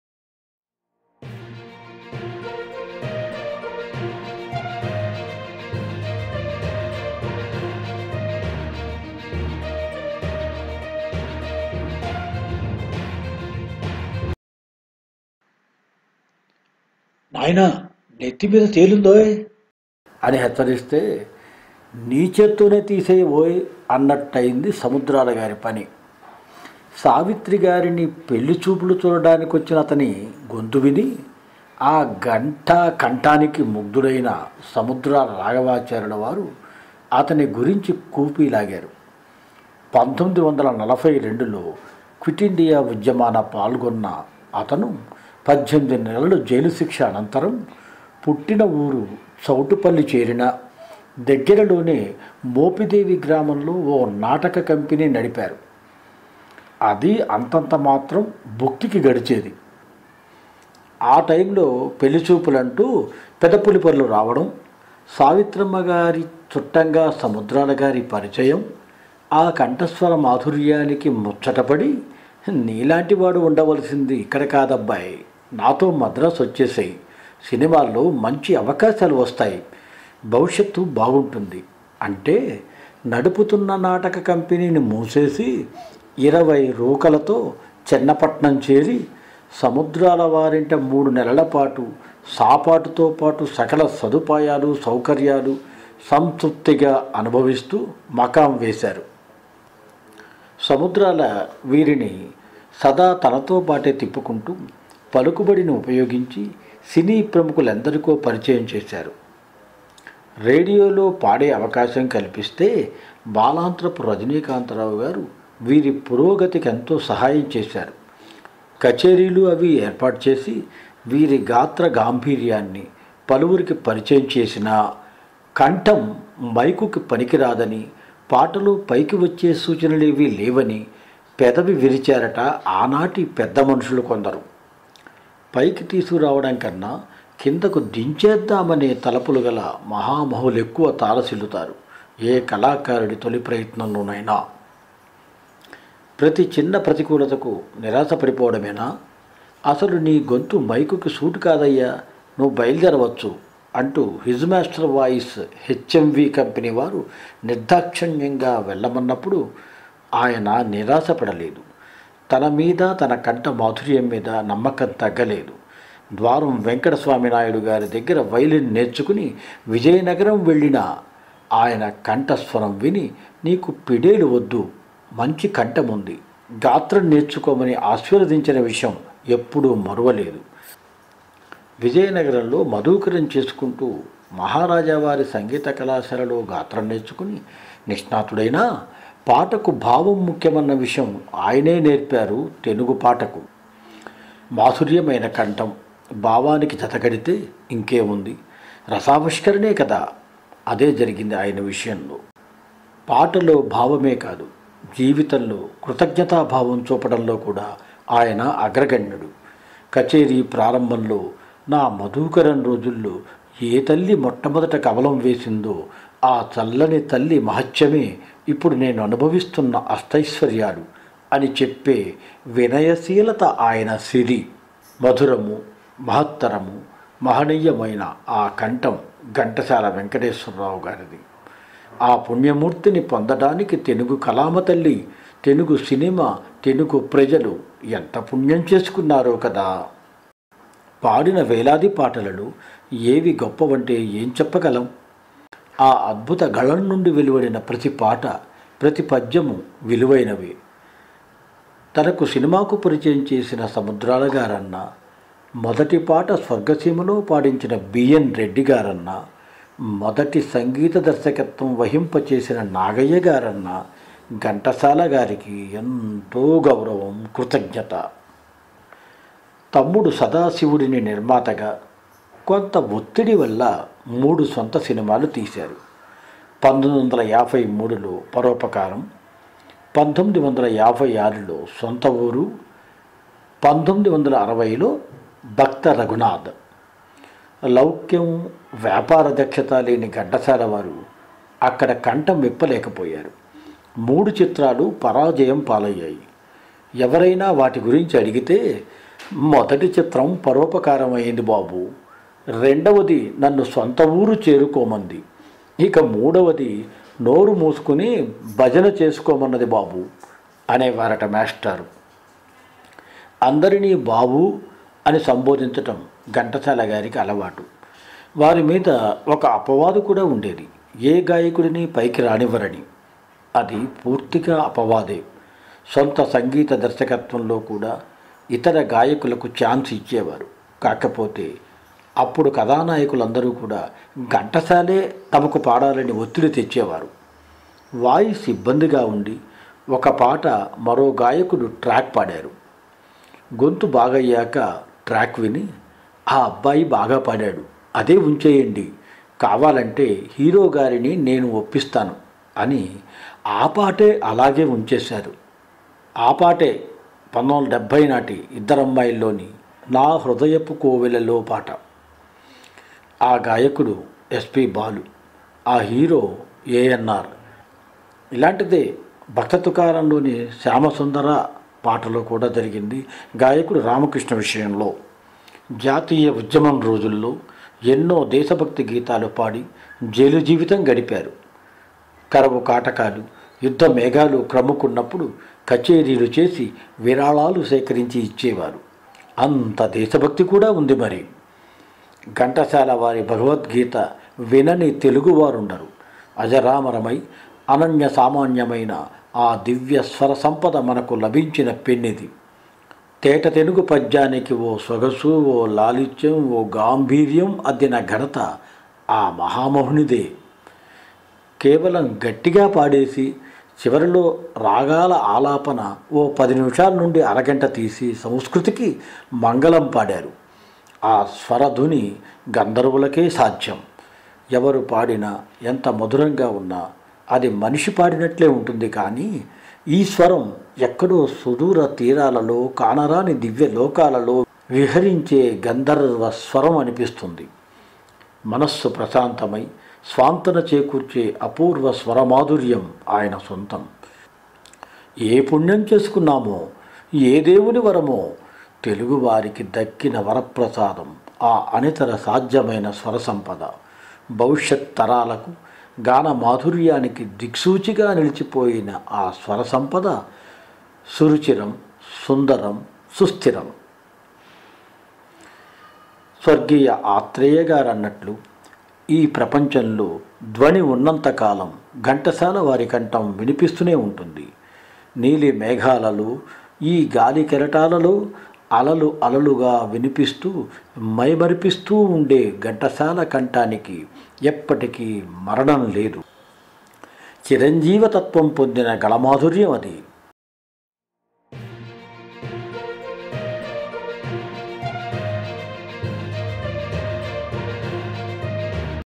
Ghantasala premanjali KVSPrasad
ఘంటసాల 98వ జయంతి సందర్భంగా డాక్టర్ కె.వి.ఎస్. ప్రసాద్ గారు ఘంటసాల మాష్టారికి సమర్పించిన ప్రేమాంజలి. (రచన :KVS Prasad)